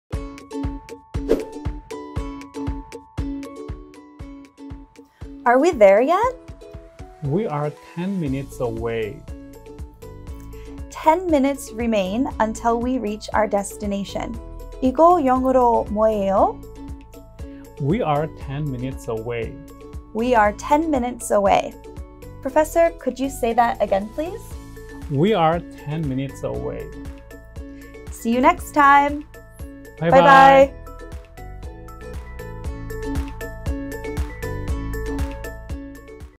[이거 영어로 뭐예요?] '10분 남았어요'
"10분 남았어요"는 영어로 어떻게 표현할까요? 영상으로 확인해보세요:)

영어전문가를 꿈꾸는 여러분들의 많은 관심 바랍니다. 감사합니다.